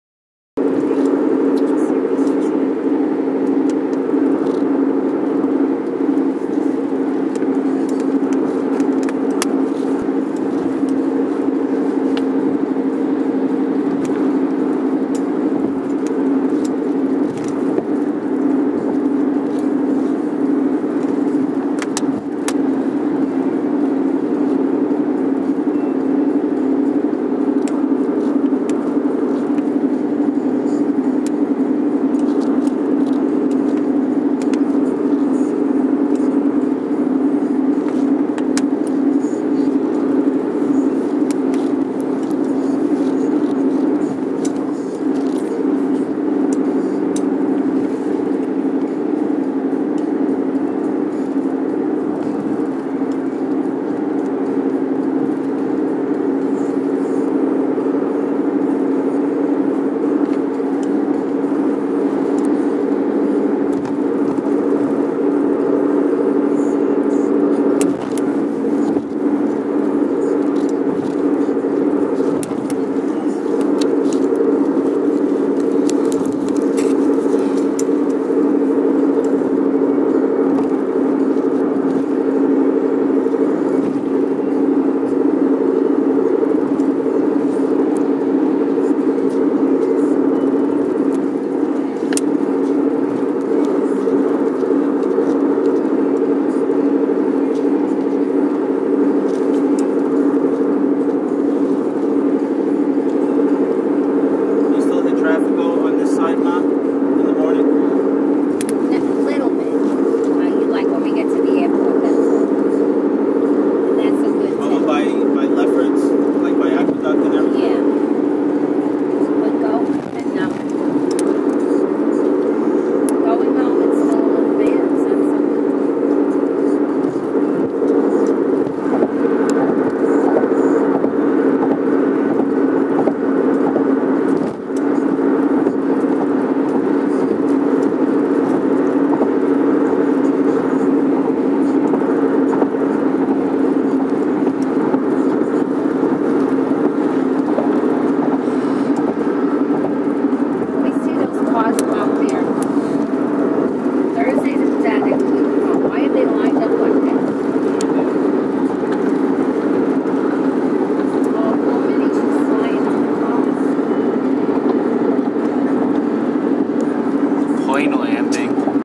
Passing By New York (JFK) Airport
1st NOTE We Were on Our Way Home from the New York Aquarium and We Drove Pass the Airport
2nd NOTE My Friend Kyle's Brother Gavin and Kyle's Mom Angela Came to New York's JFK Airport (Which Was this Airport that We Drove By Today)Yesterday Morning and Took JetBlue to California and Also my Friend Kyle Leary Will Be Driving Down to South Carolina this Weekend But he Won't Be Flying and Also Kyle is Still Home and I Still Get to See him Until he Leaves